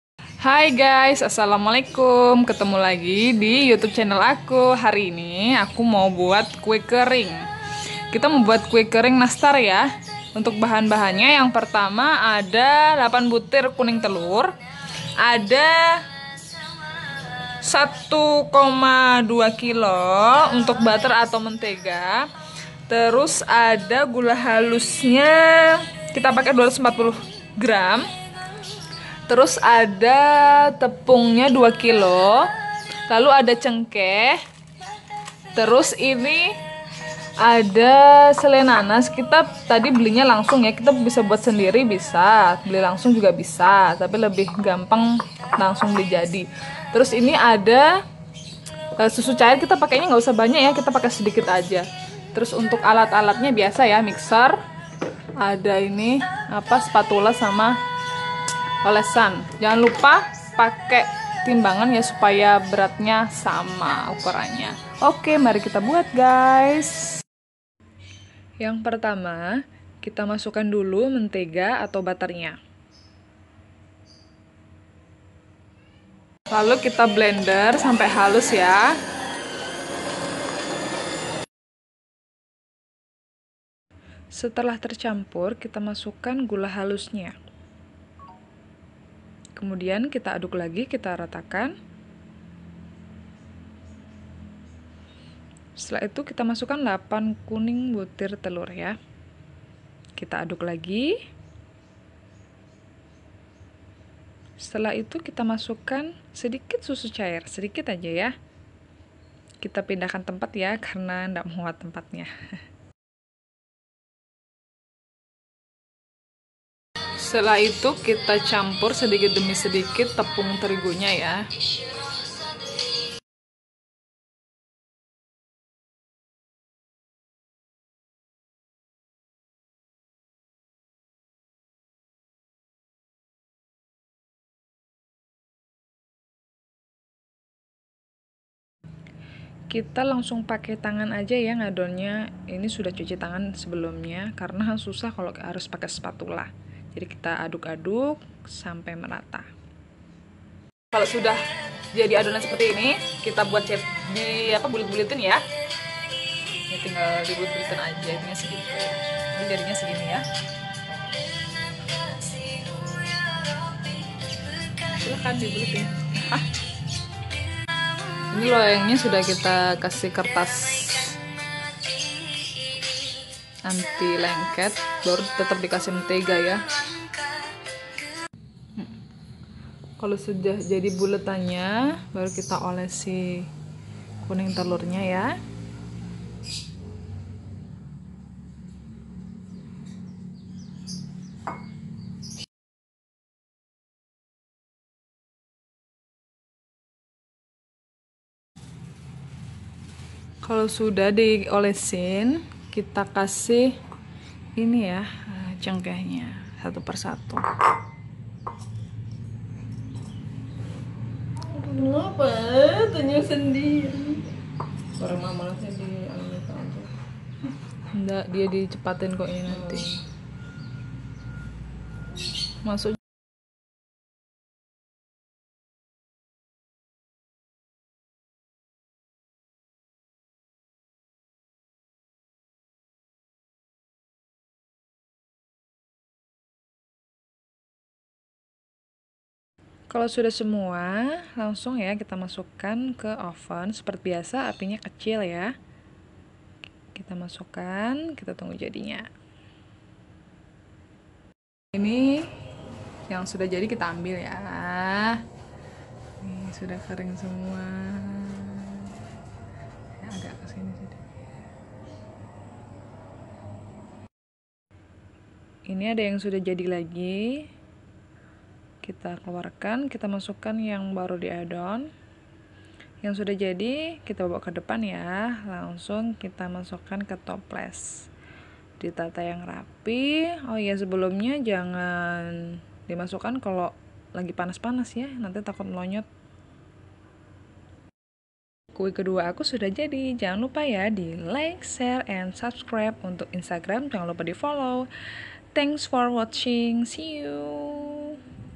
CARA MEMBUAT NASTAR PAKAI OVEN MANUAL
Bahan-bahan
📍 2kg Tepung Terigu [ Kompos ]
📍 240gr Gula Halus [ Bambu ]
📍 8 Butir Kuning Telur
📍 1,2kg Mentege/ Butter [ Simas+Hilmom ]
📍 3 Butir Kuning Telur (untuk olesan)
📍 2 Bungkus Cengkeh
📍 5sdm / Sedikit Susu Cair

Peralatan
🖋 Mixer
🖋 Spatula
🖋 Kuas

NB : Semua bahan berkwalitas medium ya guys hasilna juga enak kok, oiya bisa pakai bahan yg premium sesuaikan budget saja 😉. Lets Try Guys